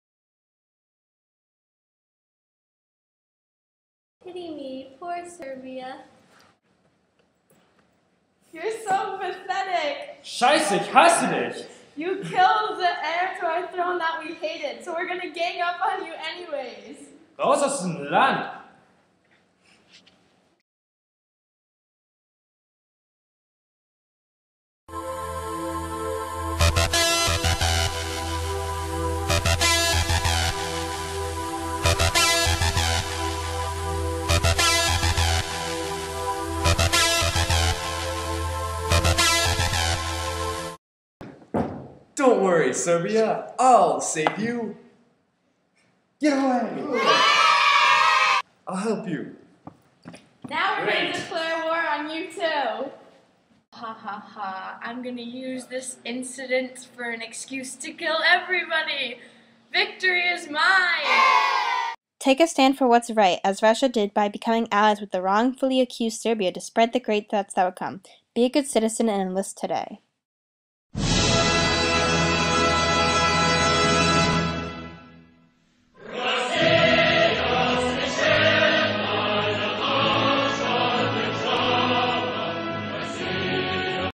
Russian WW1 Propaganda Video #2
A Russian WW1 Propaganda Video. Made by Maggie Gallogly, Julia Caffrey, and Hannah Snoeren Place.